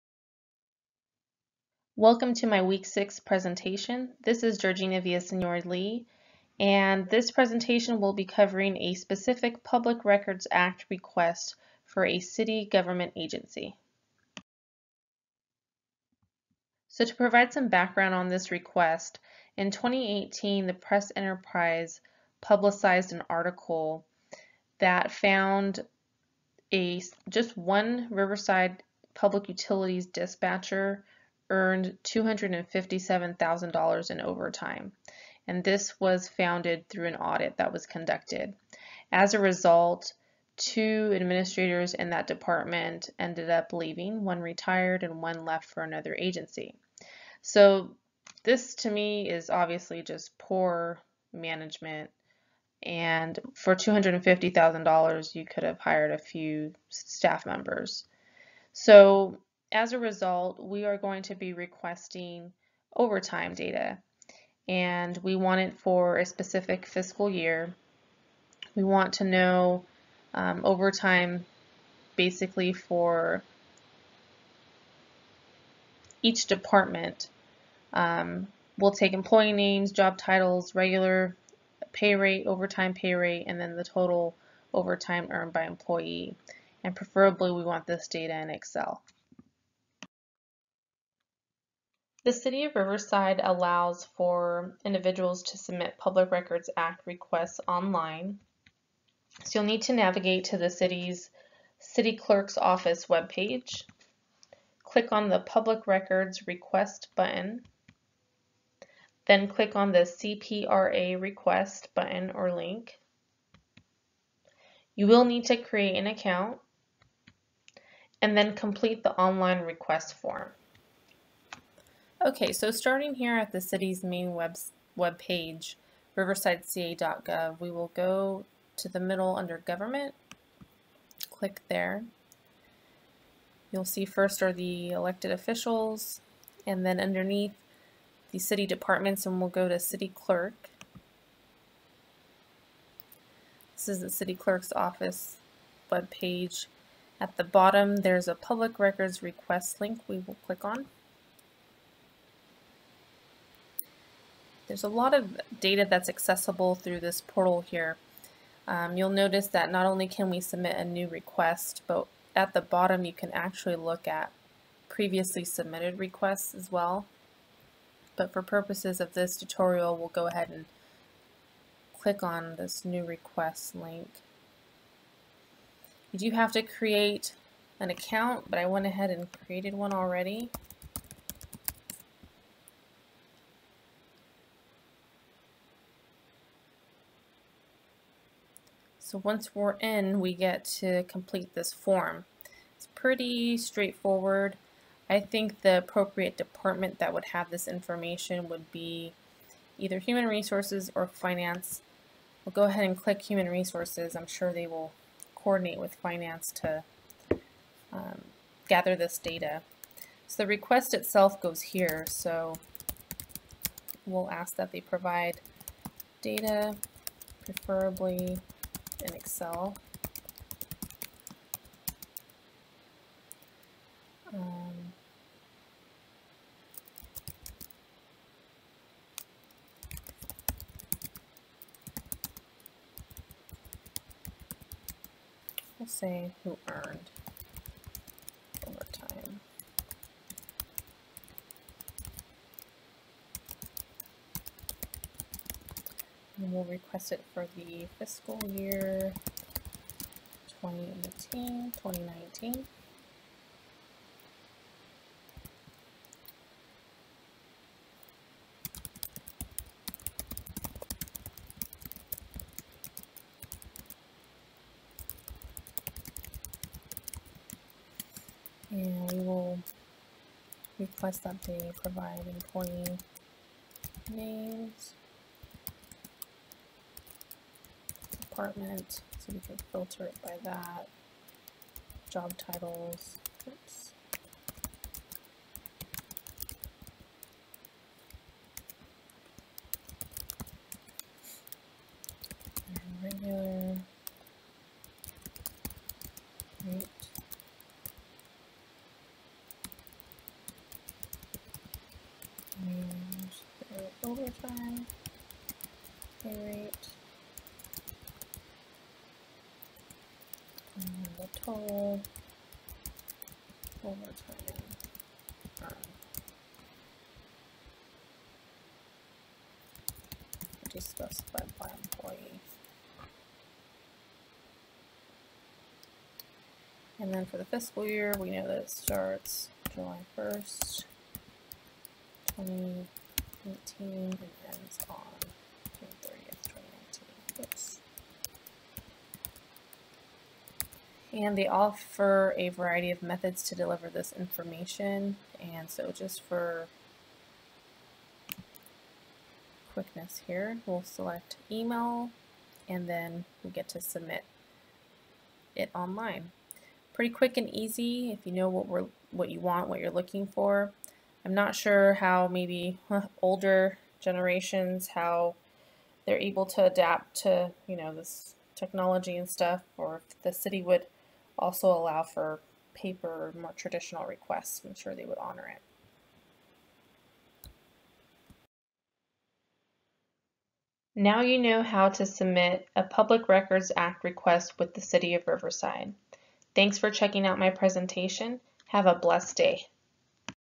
Week 6 Public Records Act Request Presentation
How to submit a PRA request to the City of Riverside